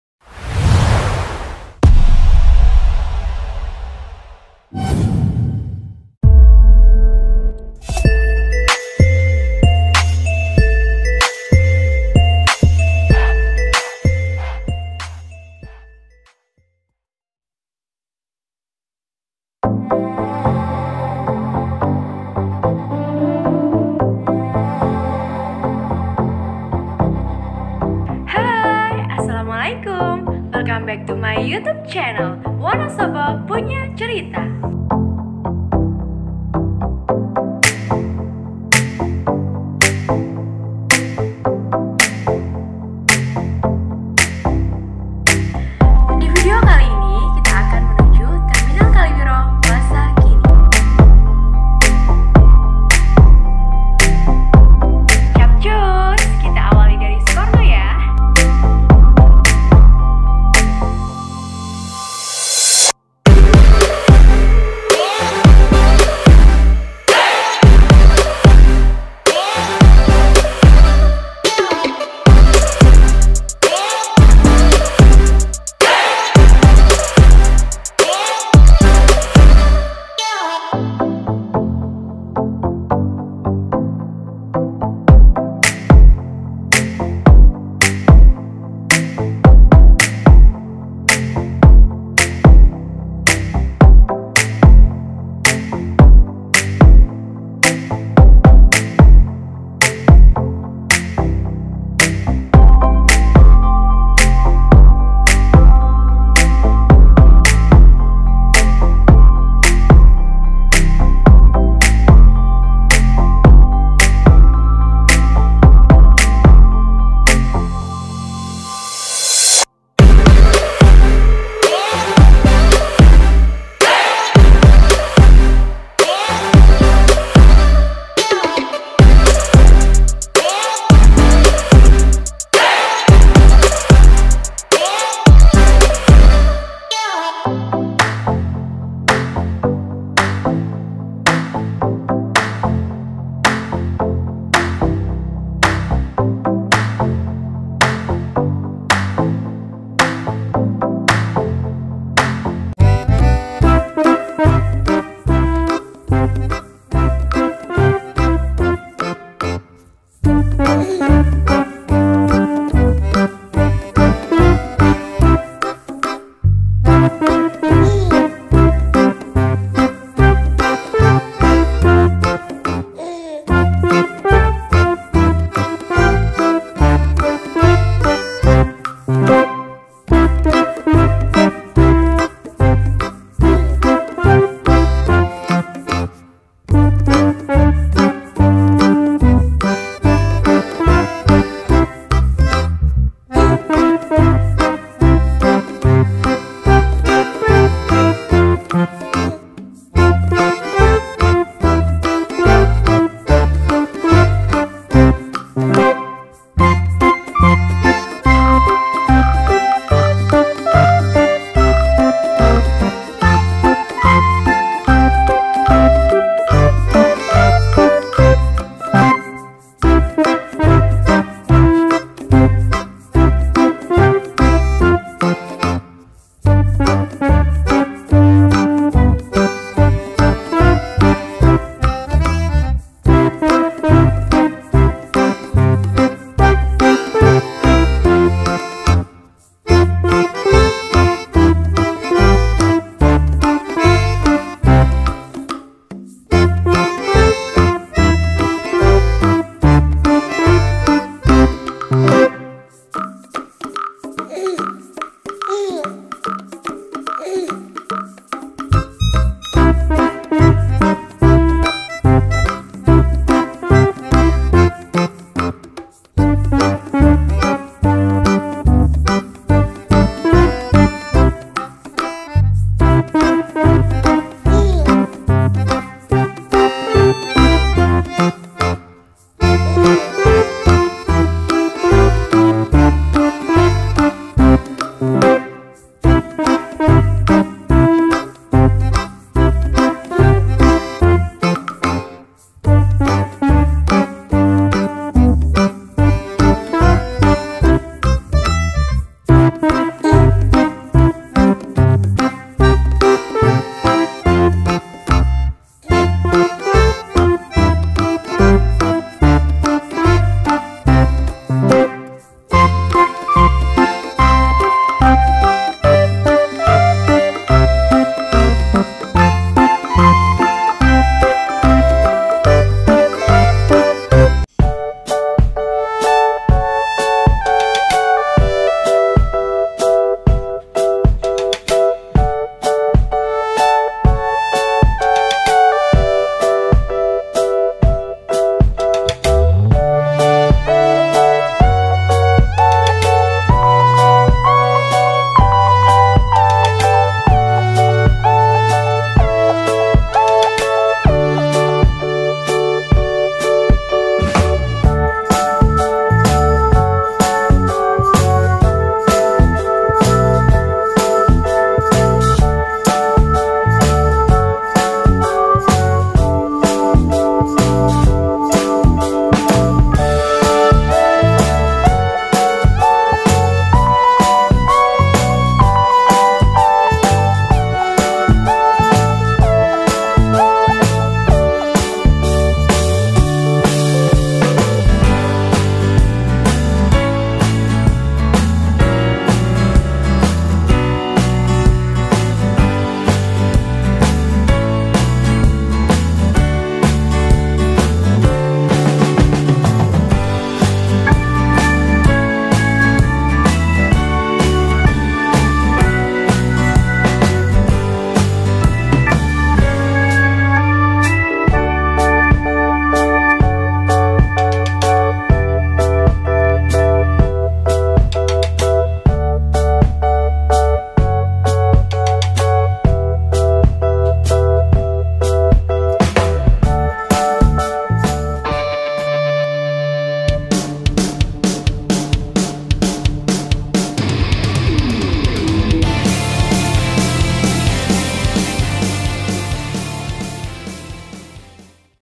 🔴KALIWIRO via SUKORENO Keren nih !! banyak bangunan LEGENDARIS ( sampai Terminal kaliwiro Wonosobo )
Alhamdulillah kali ini video tentang dari SUKORENO ke TERMINAL KALIWIRO Keren nih !! banyak bangunan baru ( menyusuri kaliwiro Wonosobo ) semoga bisa menghibur dan mengobati rasa kangen kampung halaman kita semua. Amin amin ya Rabbal alamin..
Dari Kaliwiro Wonosobo untuk INDONESIA TERCINTA.